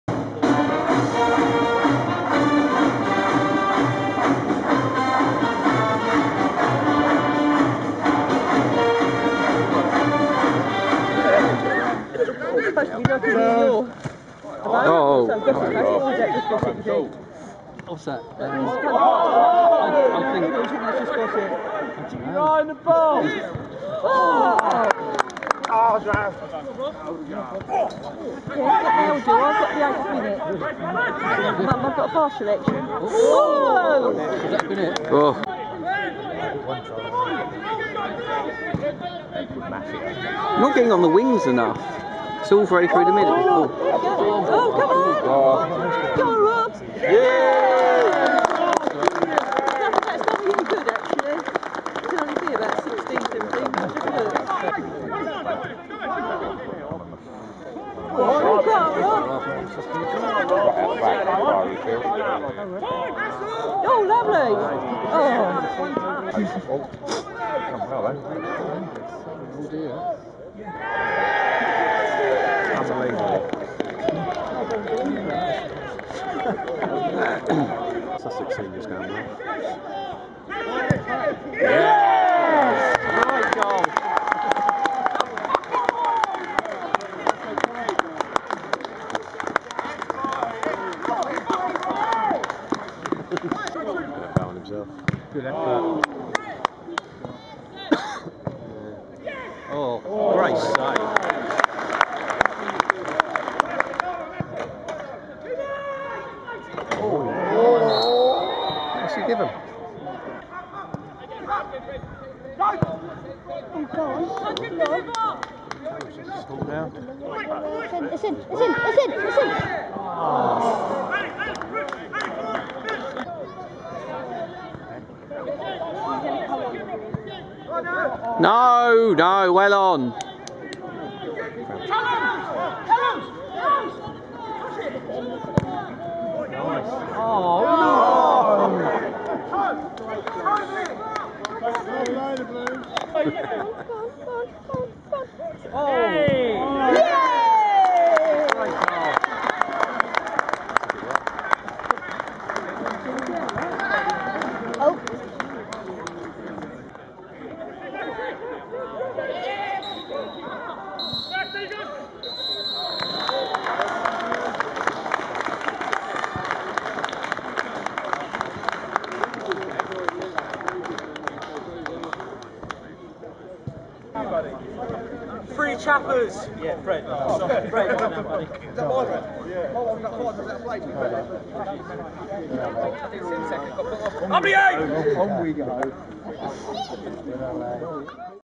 Haywards Heath Town 3-1 Oakwood FC, 28th December 2015
Highlights of Haywards Heath Town FC's 3-1 victory over Oakwood FC at Hanbury Park Stadium in the Division 1 of the Southern Combination Football League.

Heath's goalscorers were Rob O'Toole, Freddie Barker and Callum Saunders.

The win sees the team climb up to second in the table behind Crawley Down Gatwick.